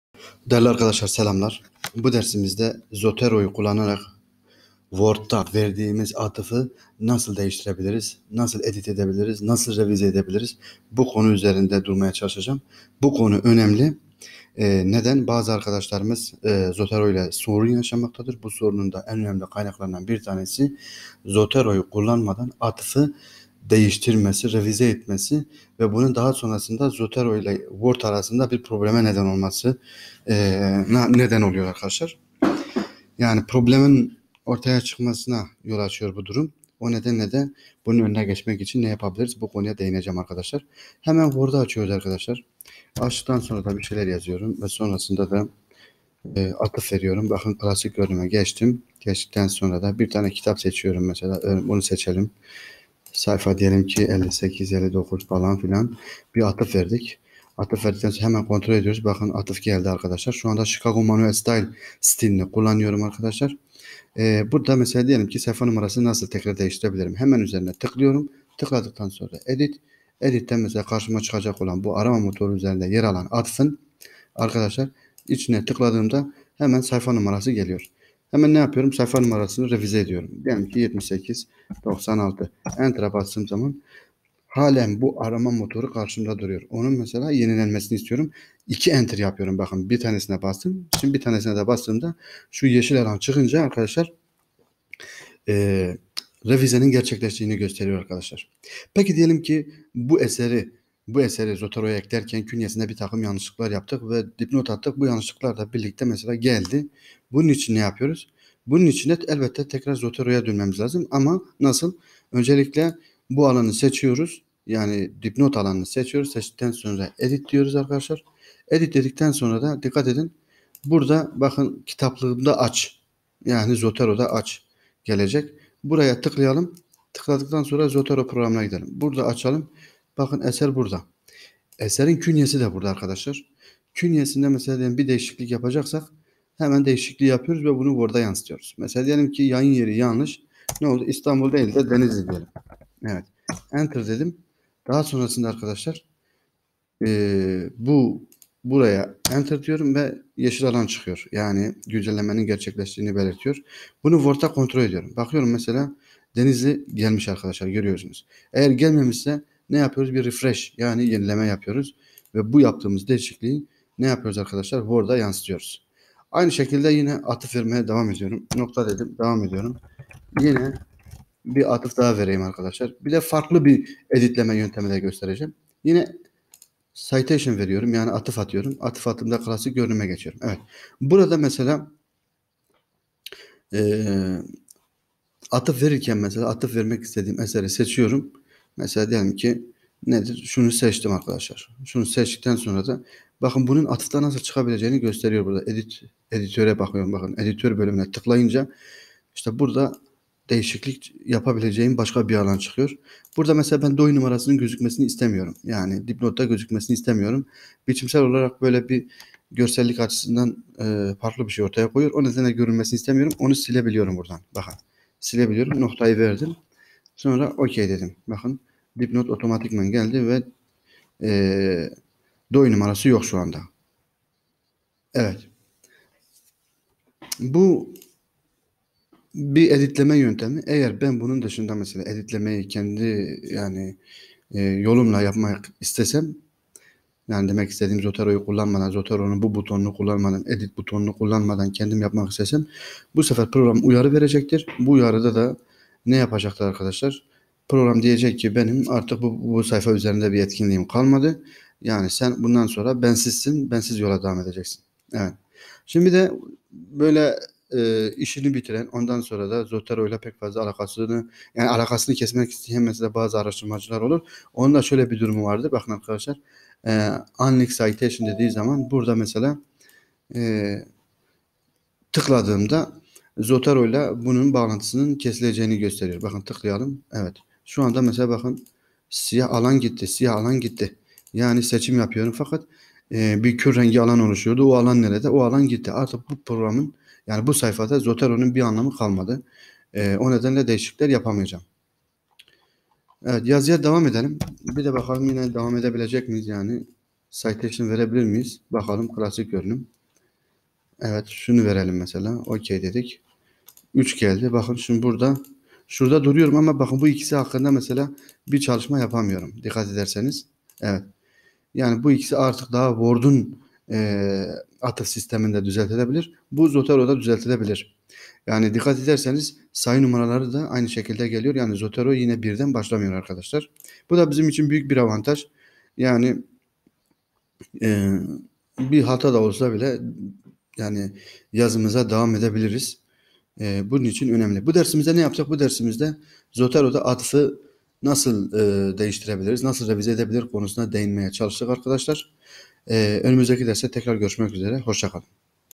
Ders 23 Zoteroda verilen atıfın revize edilmesi ve yenilenmesi nasıl olur?
diğer dersler bu video serisinde akademik yazımın bir diğer önemli programı olan zotero hakkında ders videoları hazırlayarak programın öğrenilmesini amaçladım.